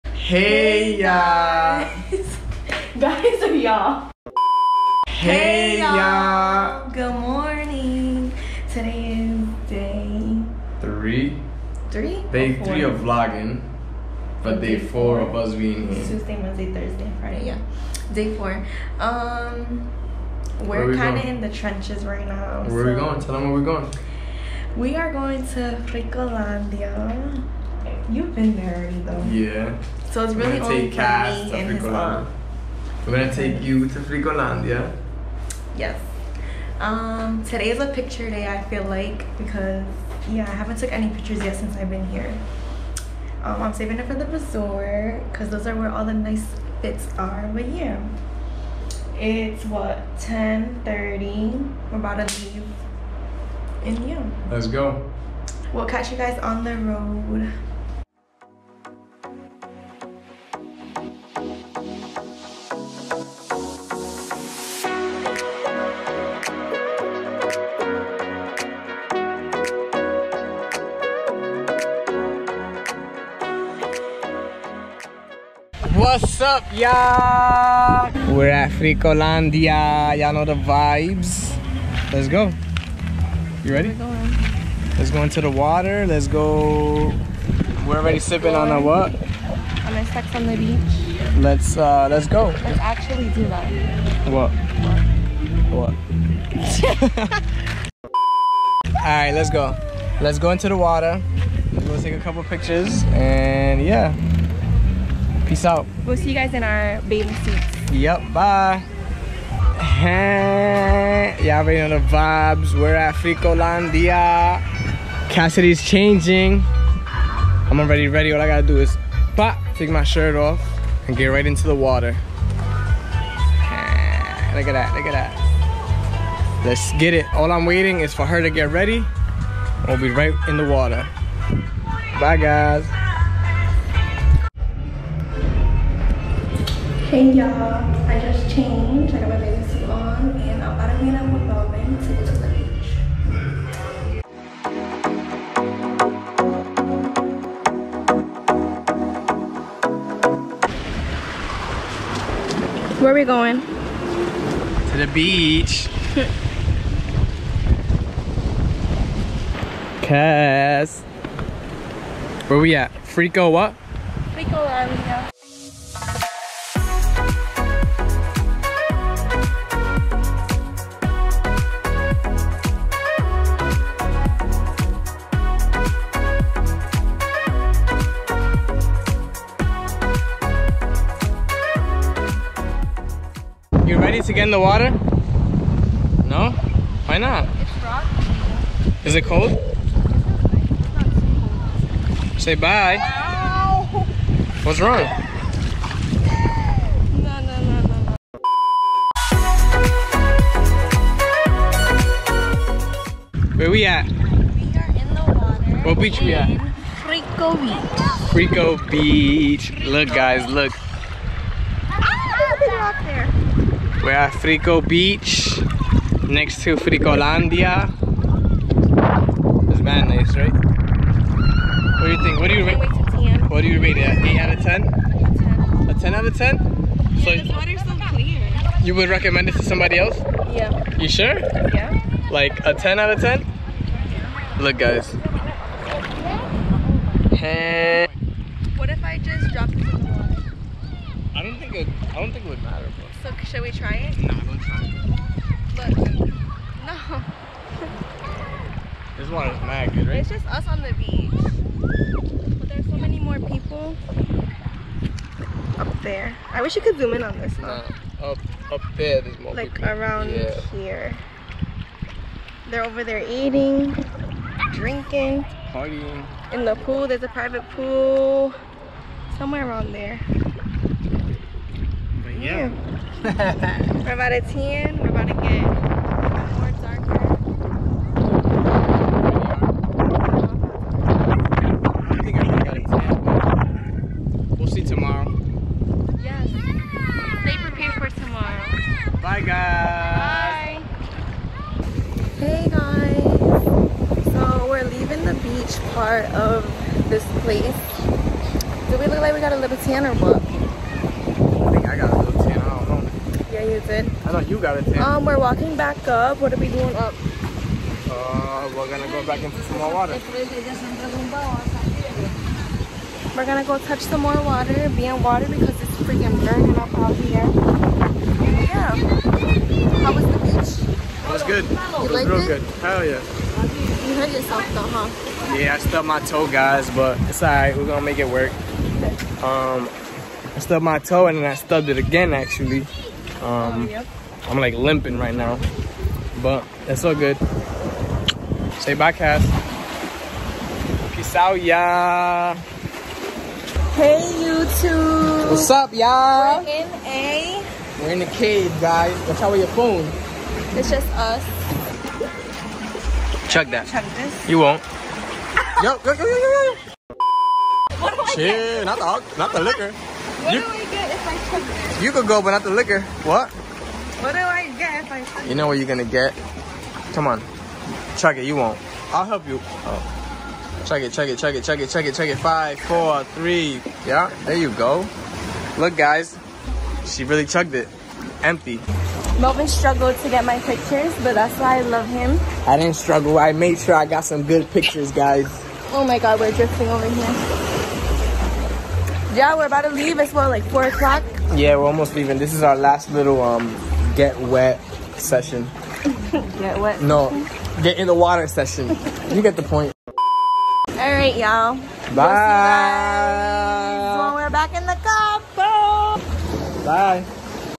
SPENT THE DAY AT FRICOLANDIA🏝️🏝️ | KASS FALLS IN WATER 🤣🤦🏽‍♂️
HEY GUYS!
Follow us on all social media down below!! 👇🏽

❌ KASSIDY’S SOCIALS ❌

❌ MELVIN’S SOCIALS ❌